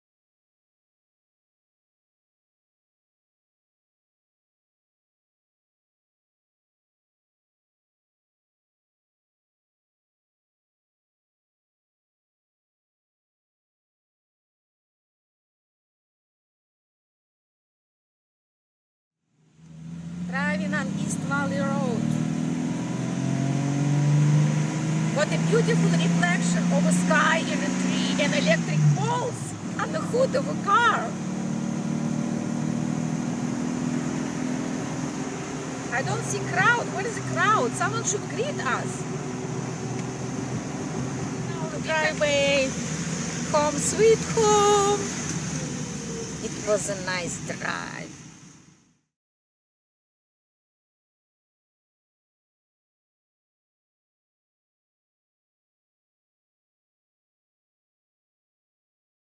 1936 Mercedes 290 Cab A restored
This started as project located in a Detroit warehouse and ended winning a trophy at Pebble Beach. Three years of hard work but worth it.